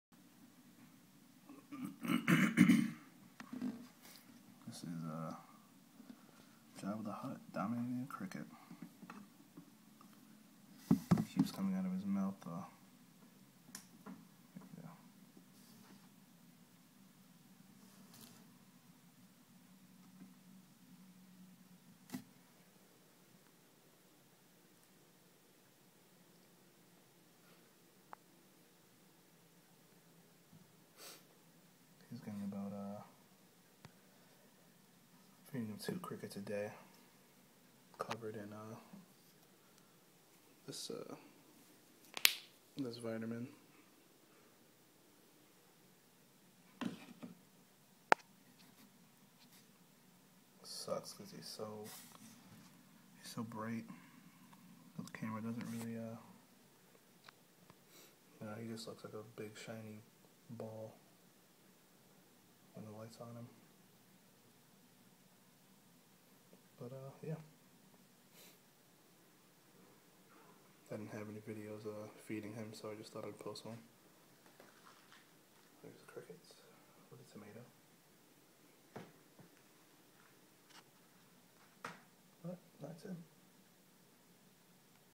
Albino Pacman Frog Feeding
Just a little video of Jabba eating.